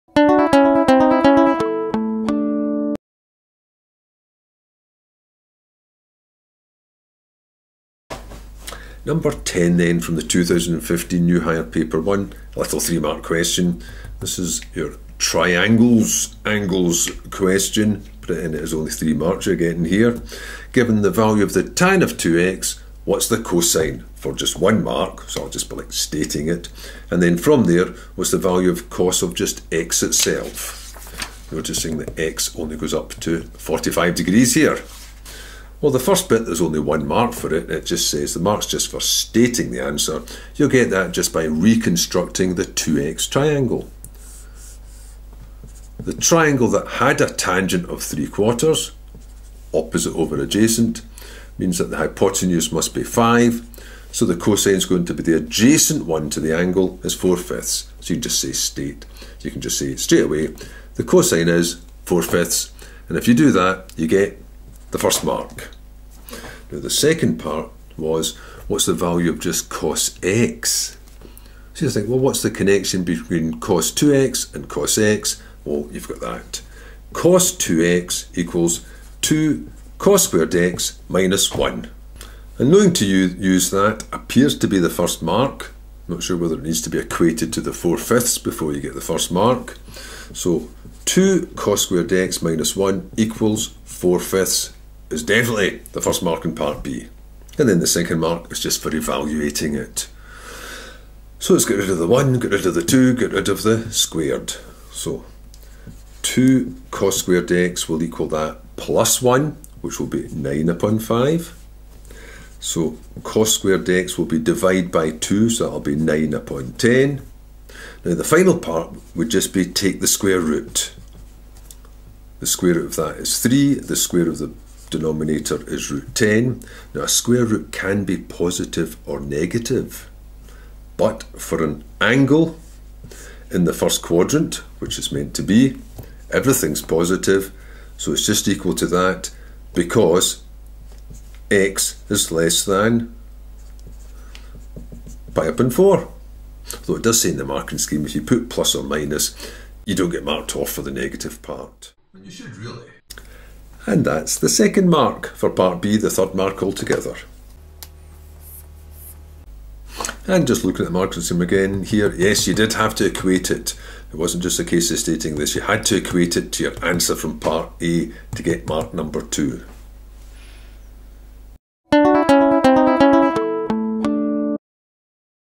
2015 SQA New Higher Mathematics Paper 1: 10 Exact trig values
Given the exact value of one trig expression, find the exact values of two related expressions.